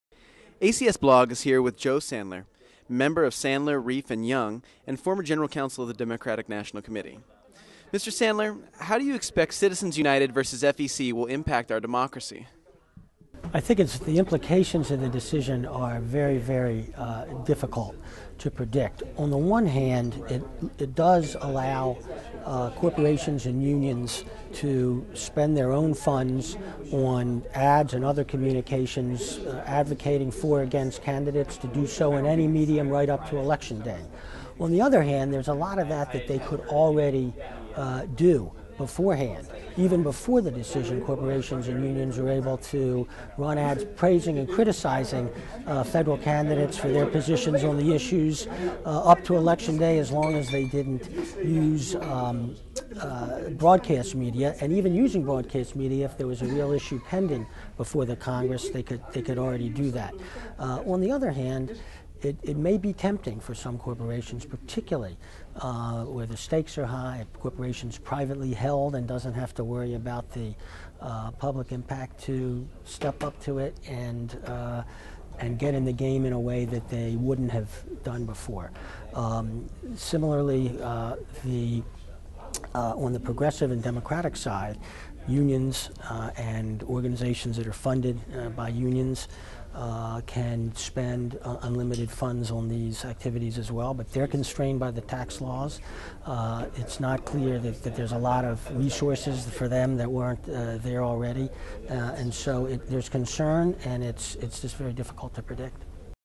Elections Law Expert Joseph Sandler on Citizens United, Elec
Following the Supreme Court decision on corporate campaign finance regulation, ACS hosted a national event exploring potential ramifications on democracy and the future of regulating corporate expenditures on elections. Joseph E. Sandler, a nationally recognized expert on elections law, joined the panel discussion and afterwards talked with ACSblog about how the decision in Citizens United v. FEC could alter electoral politics. Conceding that the decision's impact may be "very difficult to predict," Sandler said nonetheless it may now be "tempting" for some corporations "to get into the game in a way they wouldn't have done before."